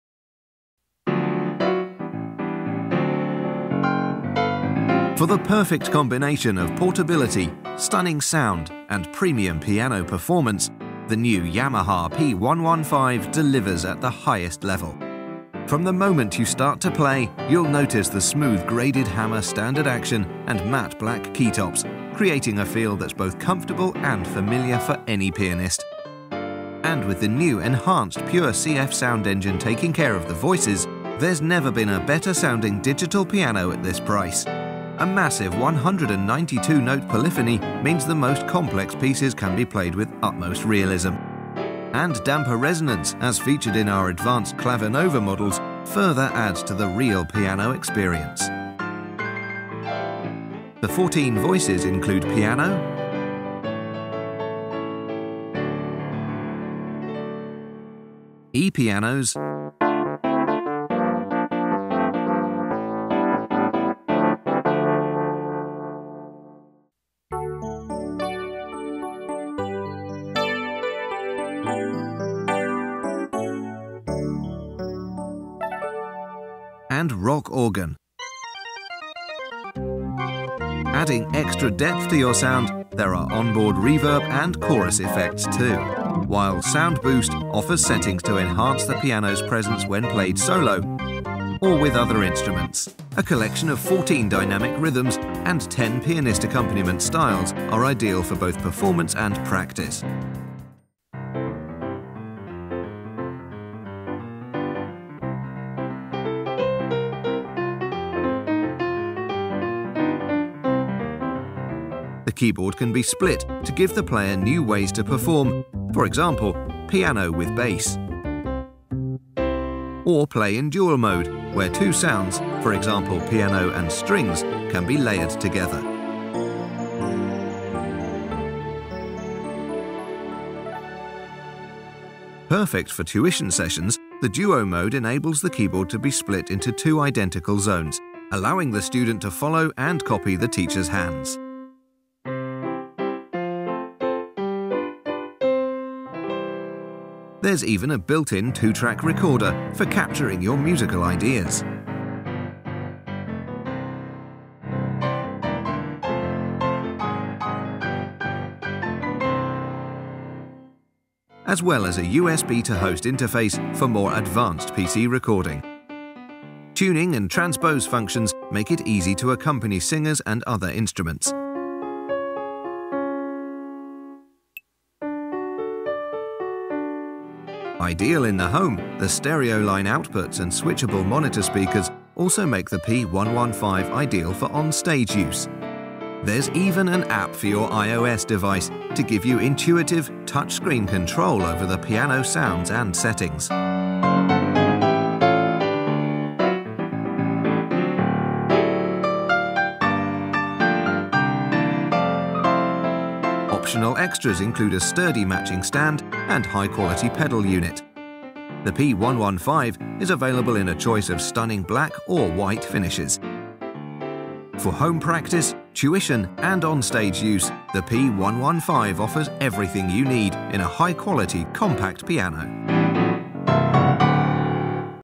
P-115 Digital Piano Overview
Explore the features of the Yamaha P-115 portable digital piano